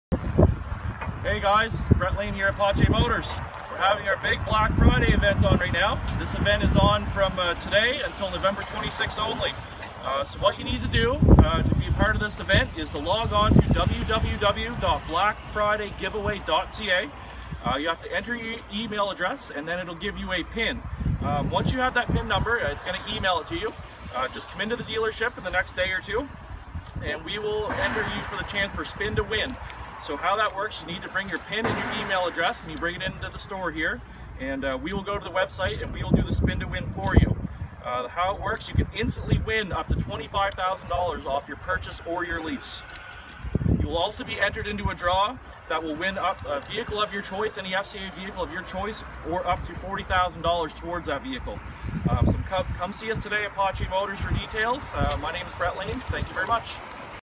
Black Friday event at Pothier Motors.
Brett Lane explains Black Friday event at Pothier Motors.